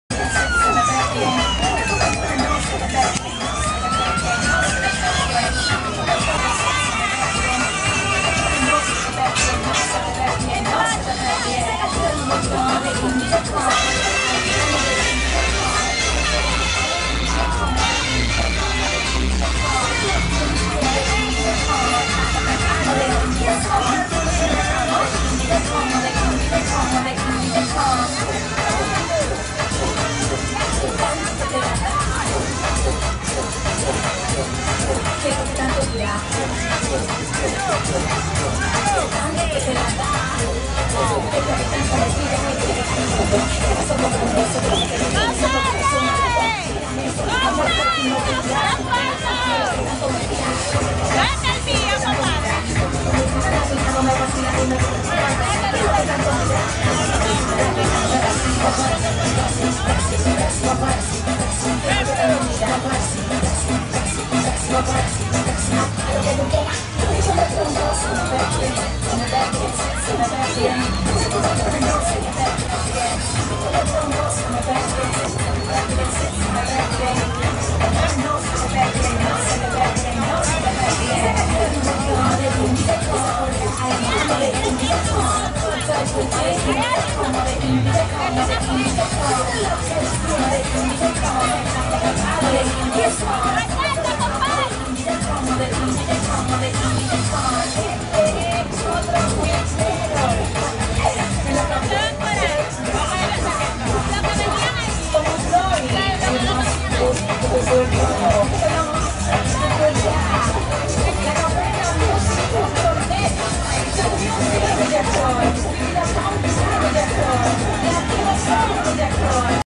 San Pedro de Macorís, desfile de Caballos
Estaba de paso por el malecón de San Pedro de Macorís y me encontré con este desfile de caballos el cual al parecer los hacen para sus fiestas patronales. Es bien relajante sentarse en este malecón a coger un fresquecito